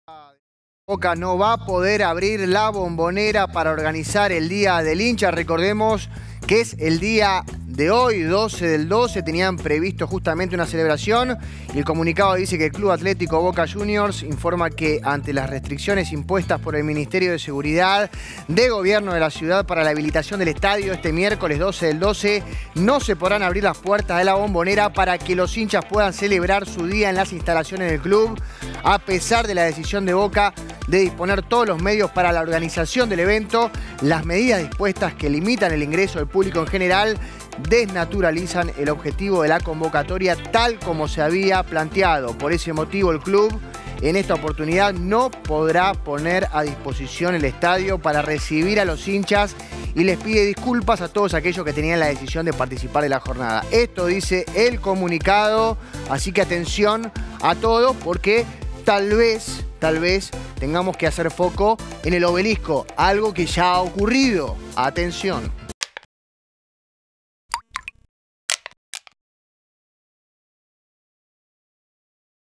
Hoy es el día del hincha de Boca Por qué se celebra
En la capital jujeña, los hinchas del xeneize se reunirán para festejar su día. Enterate donde en esta nota.